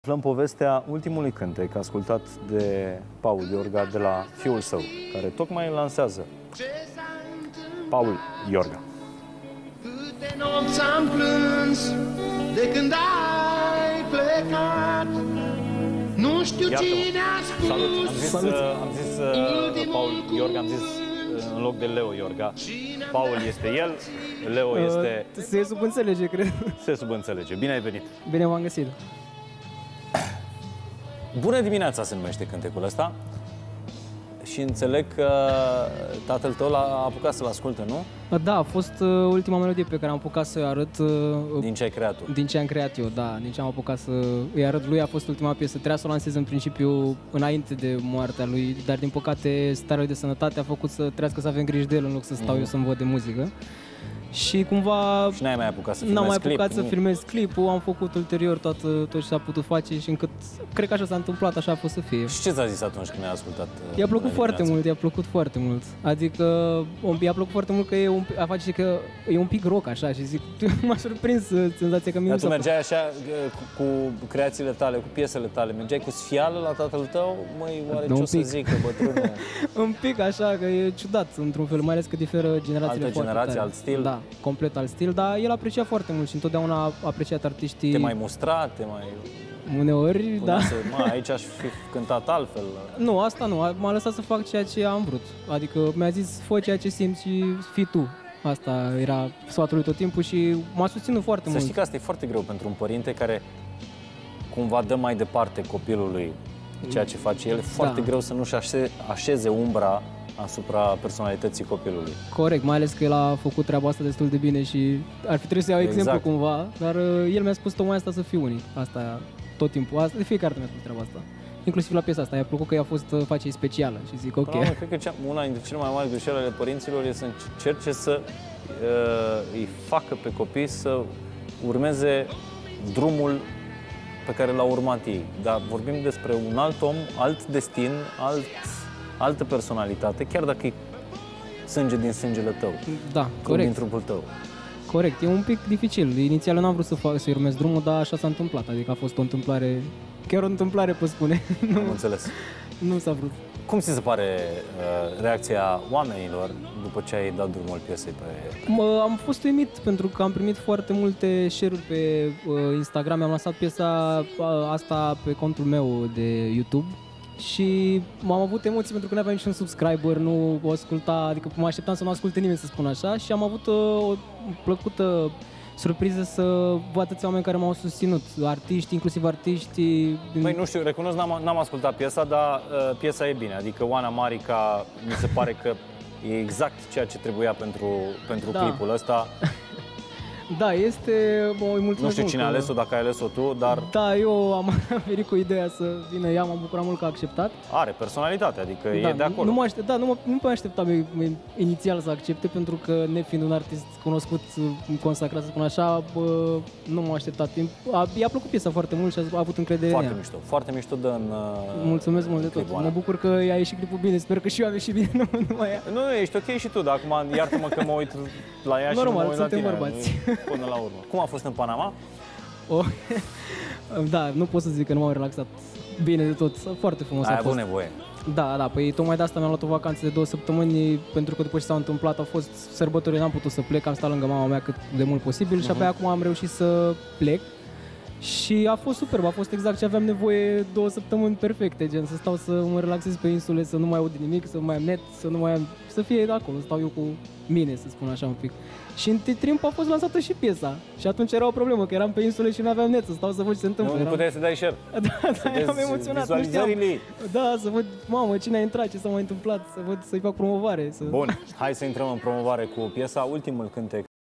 Trei luni de la moartea lui Leo Iorga. Ce decizie a luat fiul artistului
Fiul cântăreţului Leo Iorga îi duce pasiunea mai departe. Cum se descurcă Paul în lipsa tatălui, dar şi care a fost ultimul sfat primit de la regretatul artist.